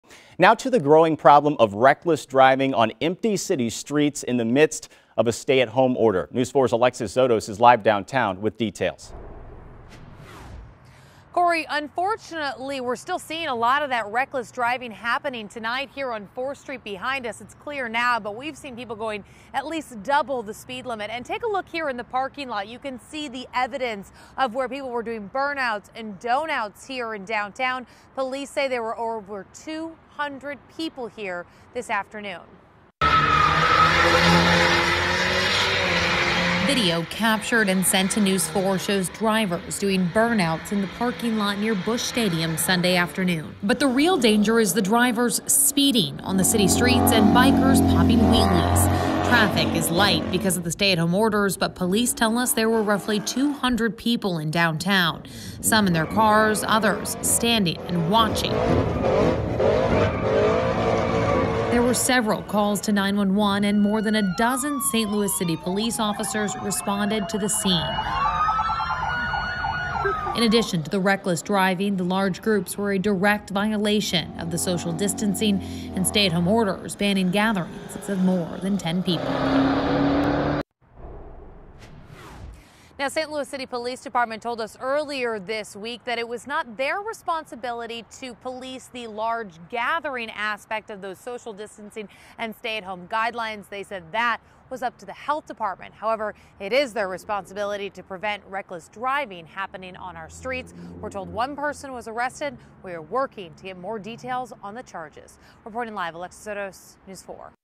Police break up crowd of more than 200 stunt drivers, spectators in downtown St. Louis
On Sunday afternoon, 4th Street in downtown St. Louis was packed full of cars and people. Videos show drivers doing burnouts in the parking lot near Busch Stadium and speeding down the street.